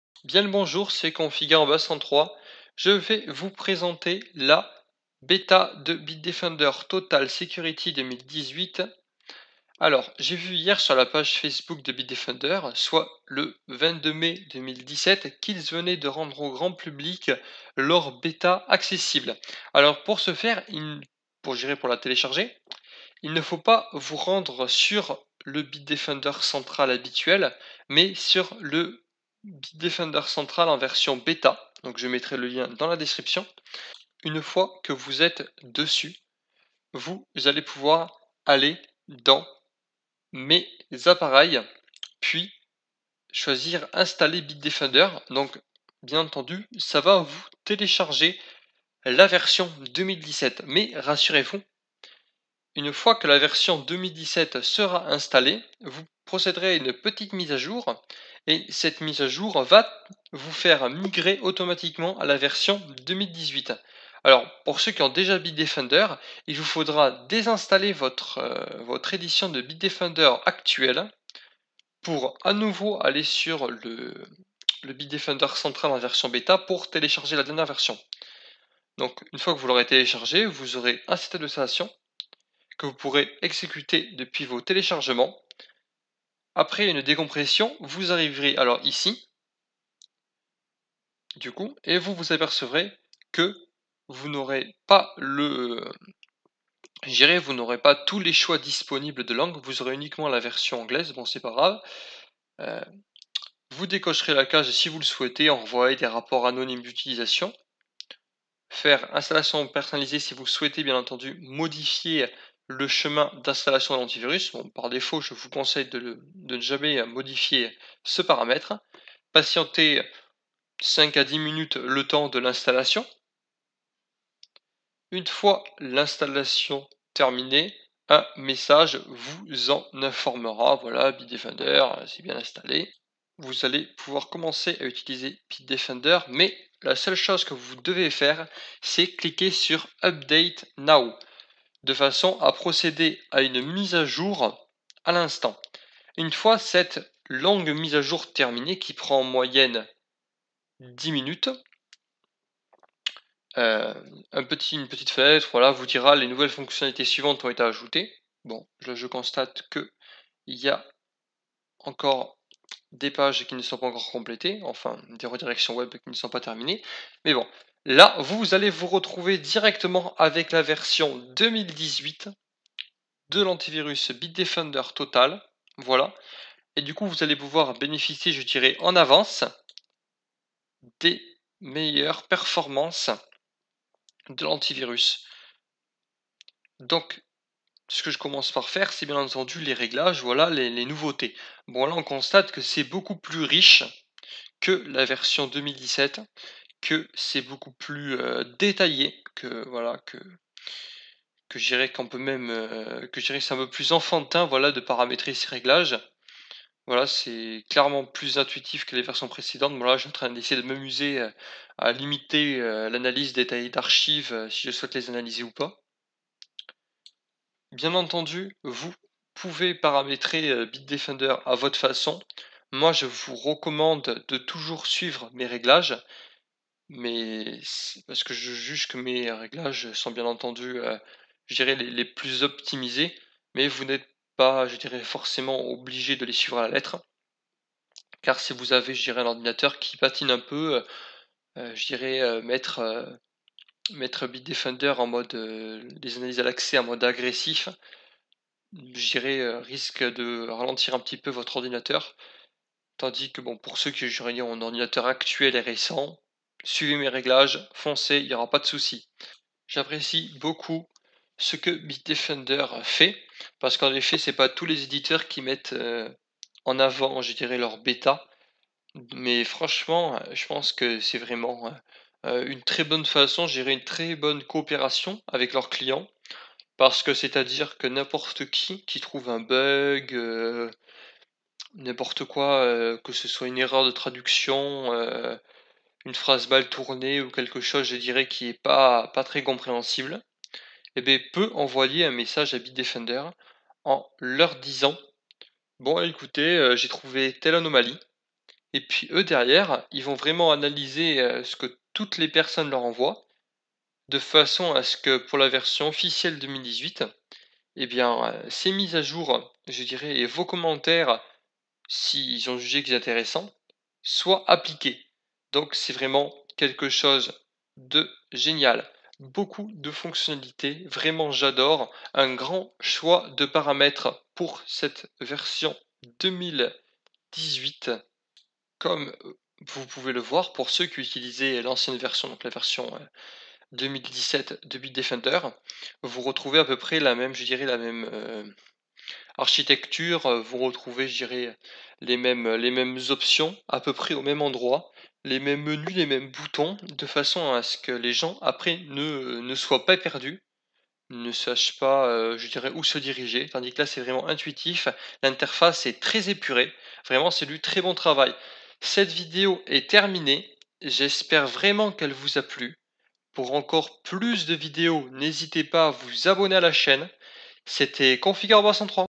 Beta de Bitdefender Total Security 2018
Dans cette vidéo je vous présente en exclusivité la version Beta de Bitdefender Total Security 2018.